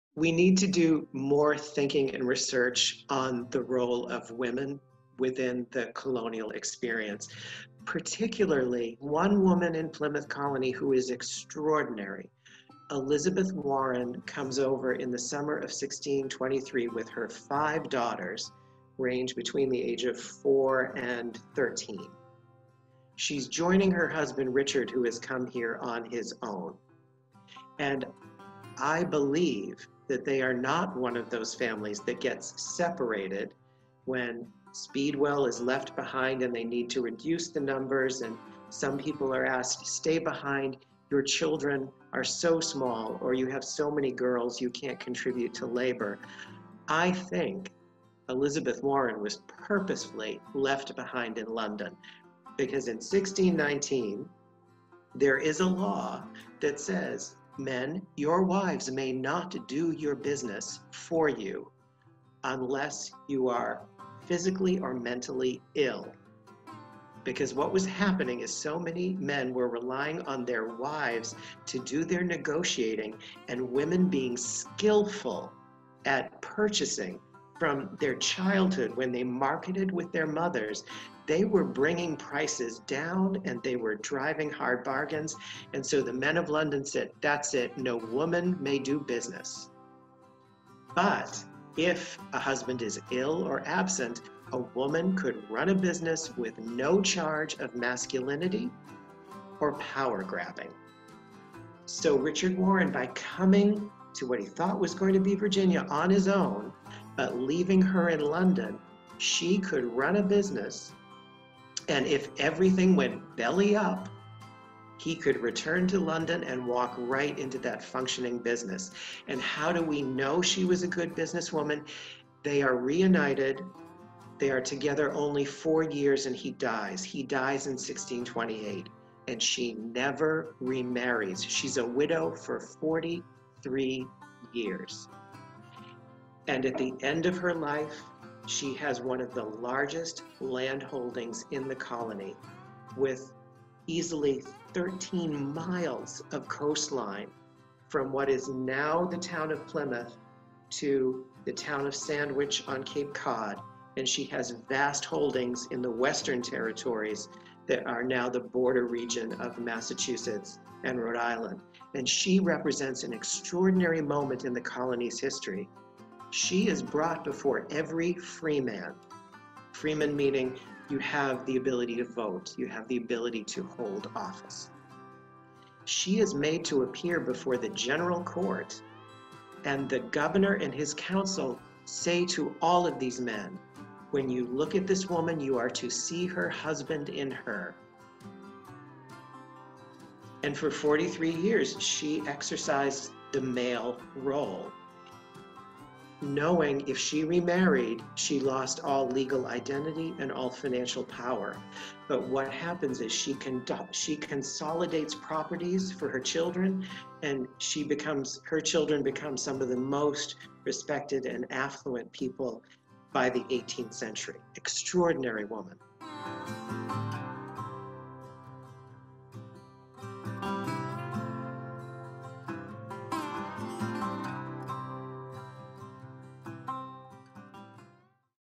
The Story of Elizabeth Warren
Four centuries ago, women played a critical role within the colonial experience.

To mark International Women's Day 2021, this short film tells the story of Elizabeth Warren, who crossed the Atlantic in the summer of 1623 with her five daughters to join her husband, Mayflower passenger Richard Warren.

Historic records show that she was a good businesswoman. After Richard's death in 1628, Elizabeth never remarried and was a widow for 43 years - knowing that if she wed again, she would lose all legal identity and all financial power.

At the end of her life, Elizabeth had one of the largest land holdings in the colony - 13 miles of coastline from what is now the town of Plymouth to the town of Sandwich on Cape Cod.

She also had vast holdings in the western territory that are now the border region of Massachusetts and Rhode Island.